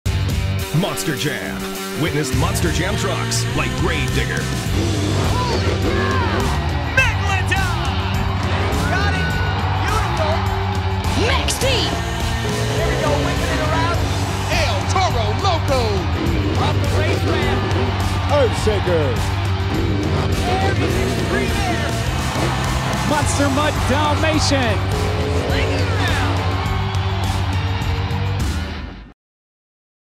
Monster Jam Races into South Africa!
South Africa! Are you ready for the fastest and loudest Monster Trucks?!

Featuring favorites such as the legendary Grave Digger®, 12-time World Champion Max-DTM, mighty Megalodon®, El Toro Loco®, Monster Mutt® Dalmatian and for the first time Earthshaker®.

Find out more at Showtime.co.za